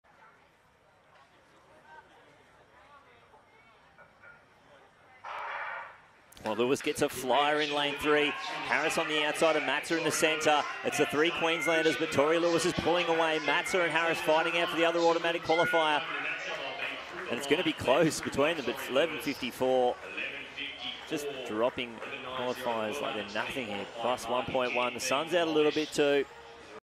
U20 Women 100m 2022 Australian Track & Field Championships
Torrie Lewis (11.52) wins Heat 3 of the U20 Women 100m Prelims at the Chemist Warehouse Australian Track & Field Championships